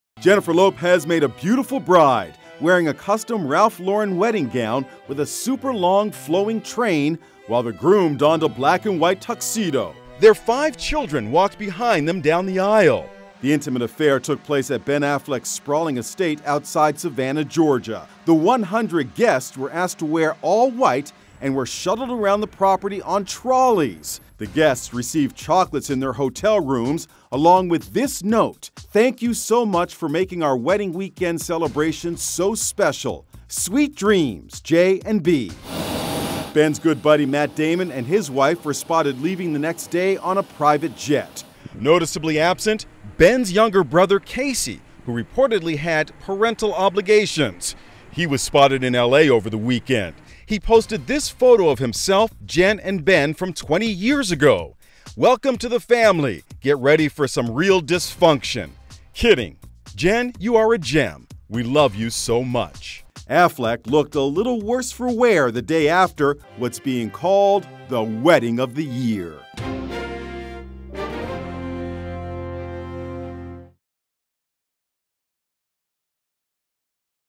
Jennifer Lopez and Ben Affleck Tie the Knot For a 2nd Time
Jennifer Lopez and Ben Affleck held their second wedding ceremony. The intimate affair took place at Ben Affleck's sprawling estate outside Savannah, Georgia. The 100 guests were asked to wear all white and were shuttled around the property on trolleys. Lopez wore a custom Ralph Lauren wedding gown with a long flowing train, while the groom wore a black and white tuxedo. Their five children also walked behind them down the aisle. Their guests received chocolates along with a thankful note.